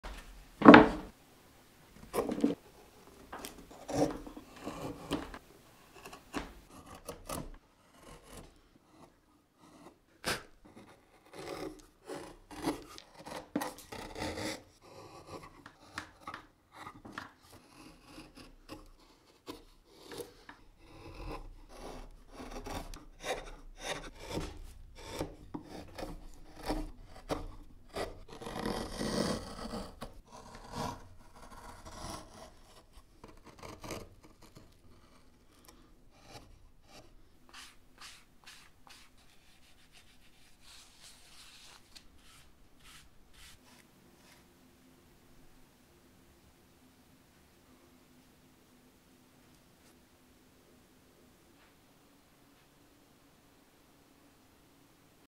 Hand Carving HOLLOW KNIGHT
How I made Hollow Knight out of a block of basswood.

~ Schaff Wood Gouges
~ Diamond Sharpening Stone
~ DEWALT Jig Saw
~ Tried & True Danish Oil